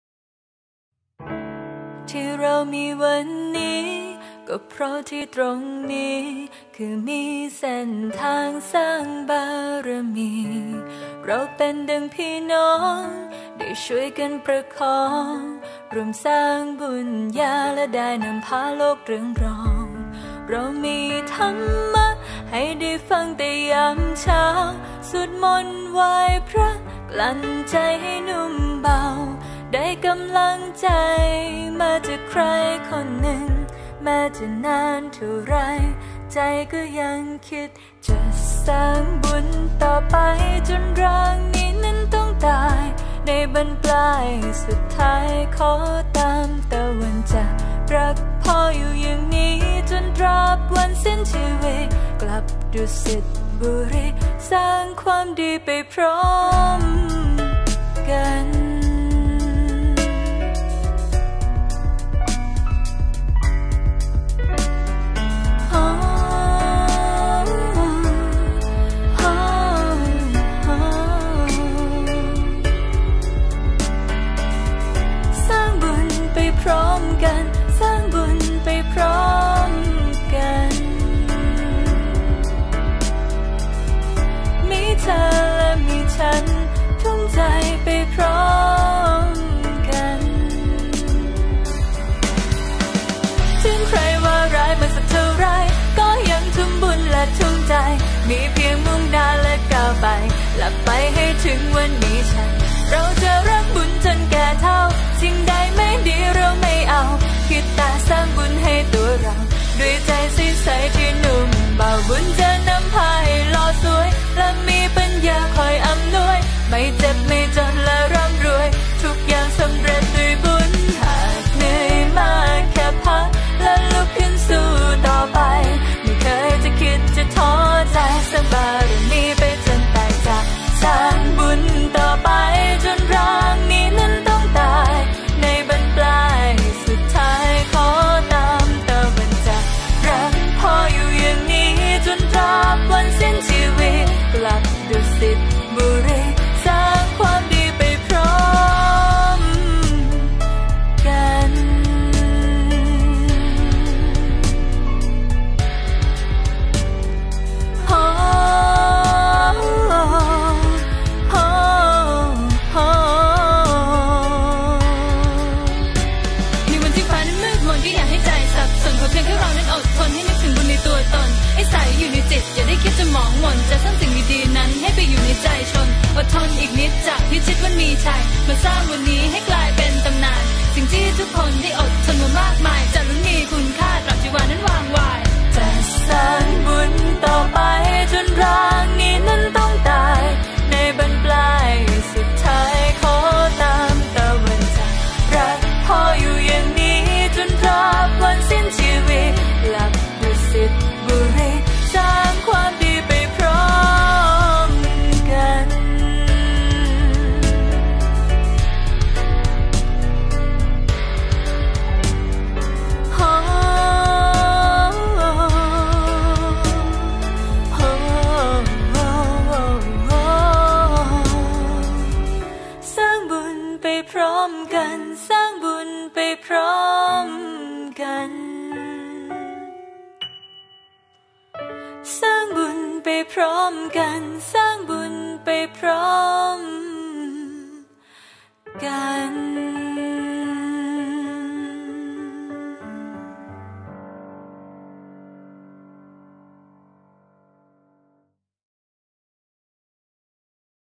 9th Anniversary of Dhammakaya Meditation Center Boston Celebration Ceremony, August 26th, 2018.
พิธีบุญฉลองวัดพระธรรมกายบอสตัน ครบรอบ 9 ปี ณ วันที่ 26 สิงหาคม 2561.